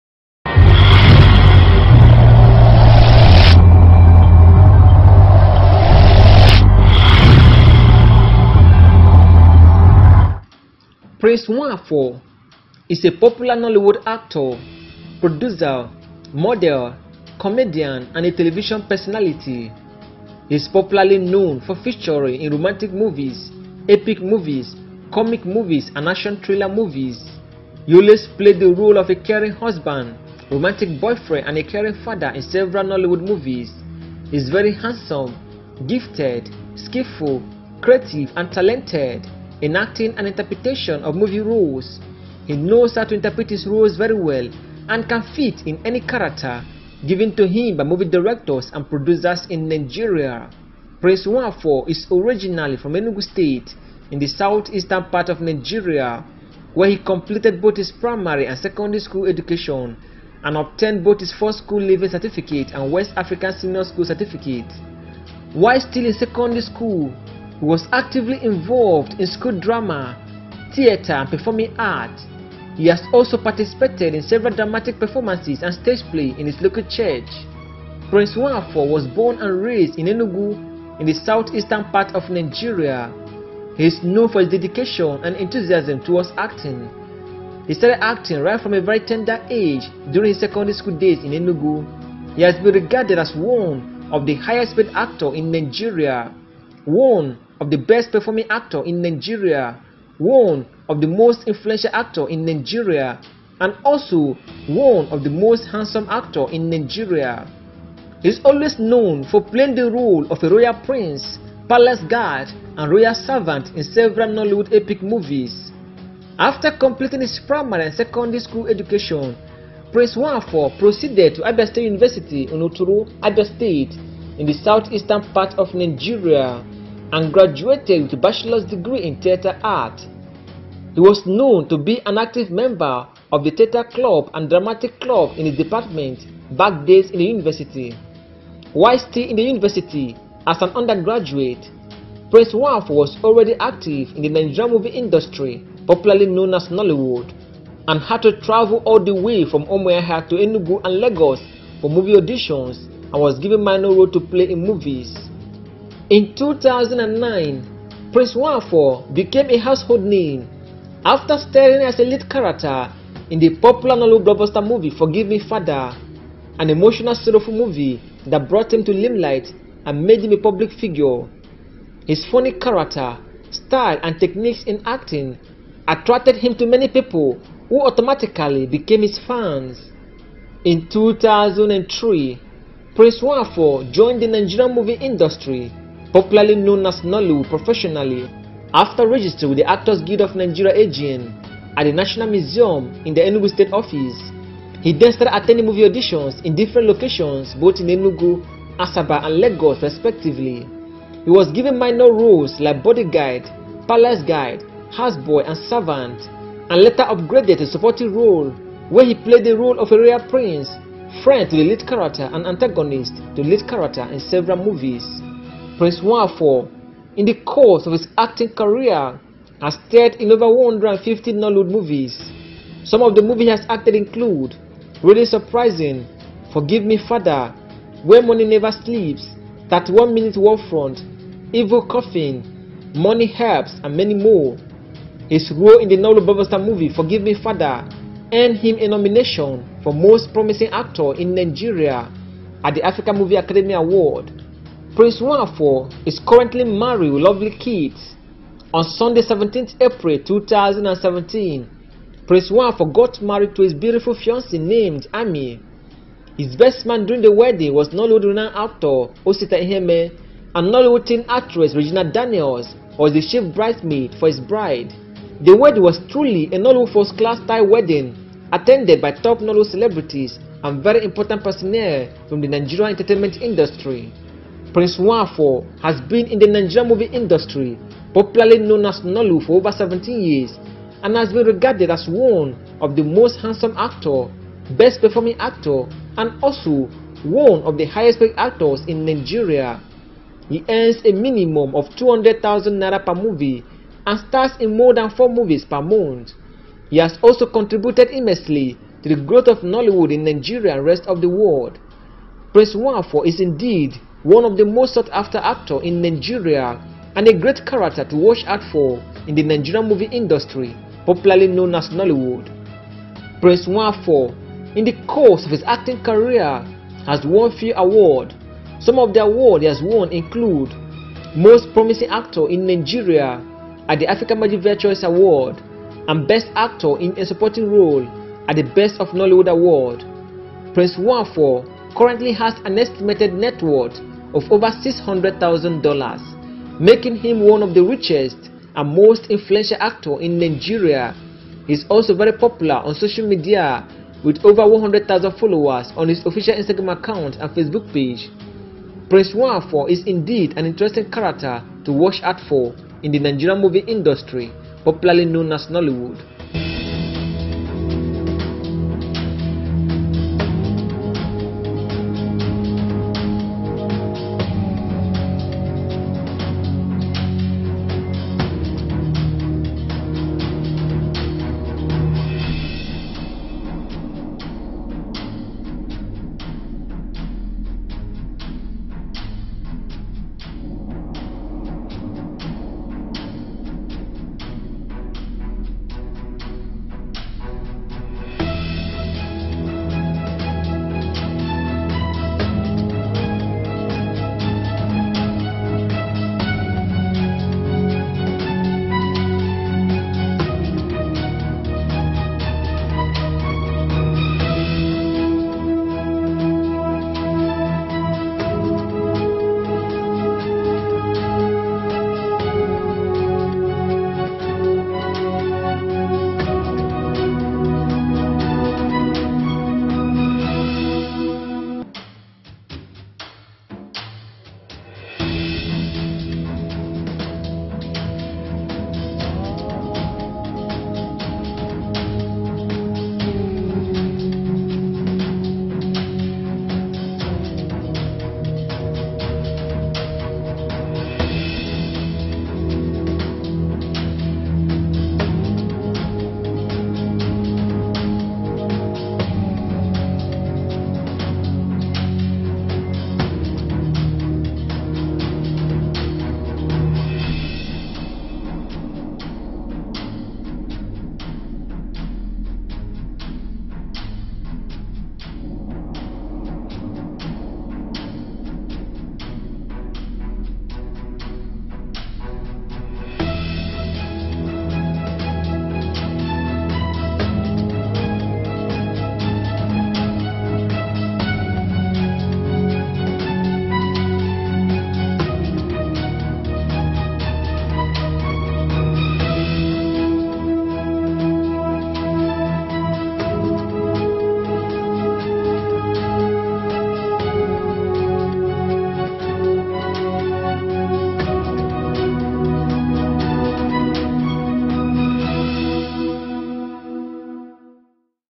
Prince Nwafor Biography, Age, Children, Family, Lifestyle & Net Worth
In this video I will be sharing with you the biography, age, children, family, lifestyle and net worth of Nollywood actor Prince Nwafor. I sincerely believe you will be entertained, informed, inspired and captivated with this channel.